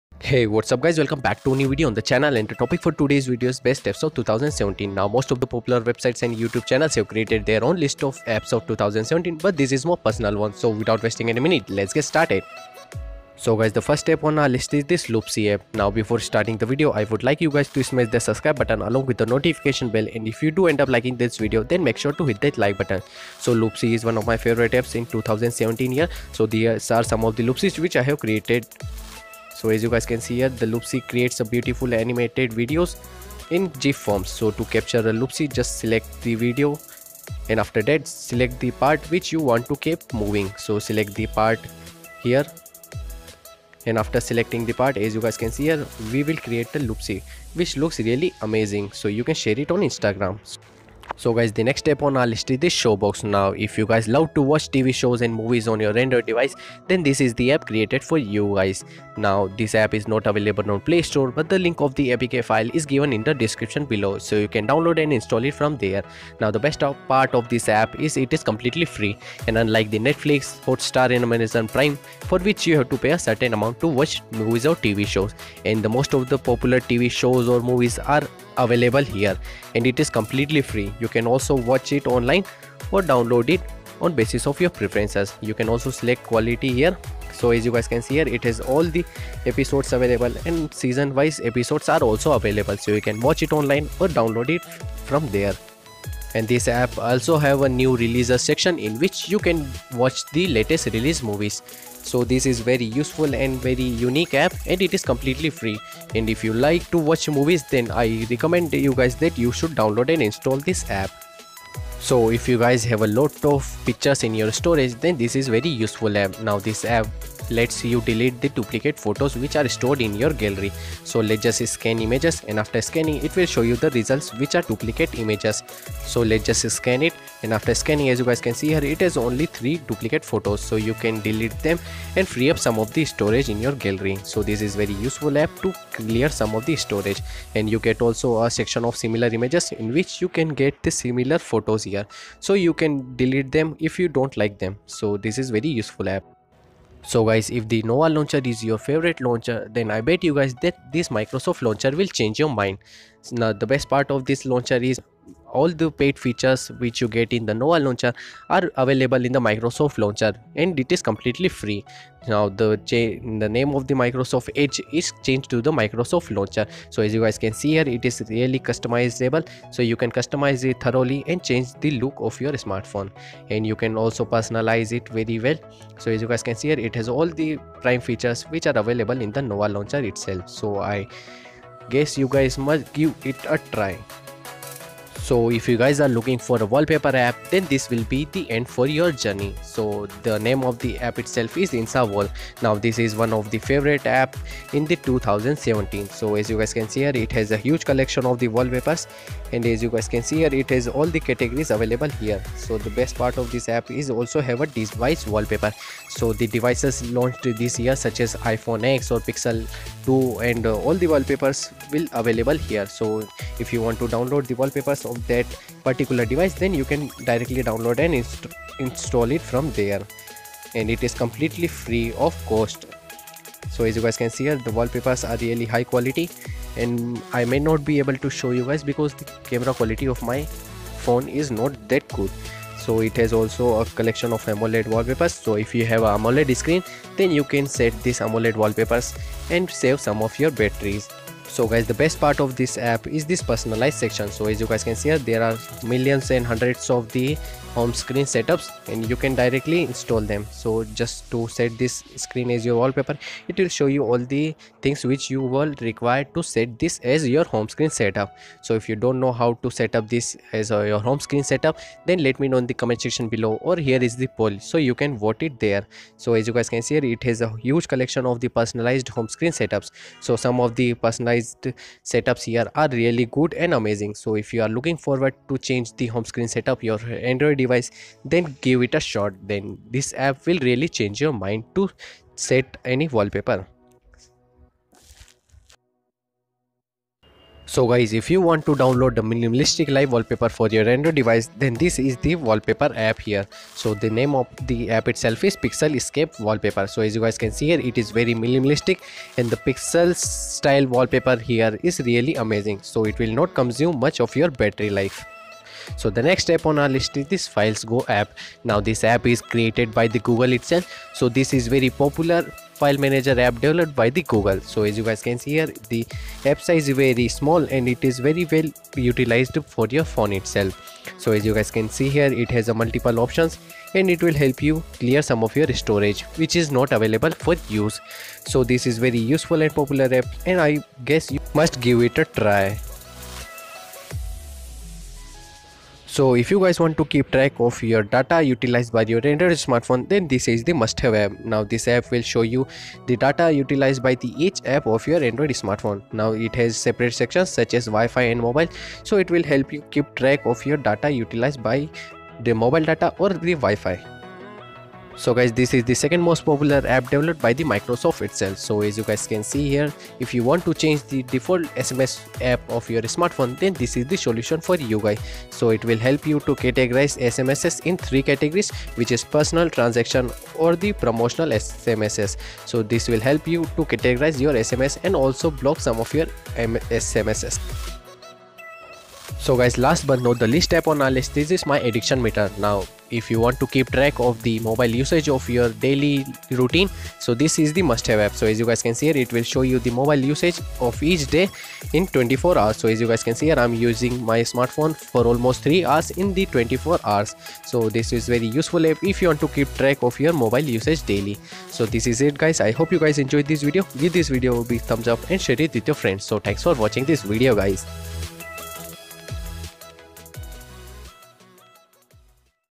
Best apps for Android 2017 (New research)
Best apps for Android 2017.
Every year both the Android and iOS platform releases  millions of new apps. There are a few apps which are worth your time. I try out many new apps to recommend the best of them to you guys. Now that 2017 is coming to an end, it would be a good time to publish the list of best apps of 2017. The list has been created based on my personal experiences so I really hope that you will enjoy them. So, get your phones out and check out these best apps for Android for 2017:

Time stamp for the video :-

1 Loopsie (0:13)
2 Show box (0:51)
3 Remo duplicate (1:52)
4 Microsoft launcher (2:28)
5 Inswall (3:06)
6 Pixelscape (4:48)
7 Filesgo (5:06)
8 Glasswire (5:36)
9 SMS organiser (5:56)
10 My addiction meter (6:18)

Download link of all the featured apps :-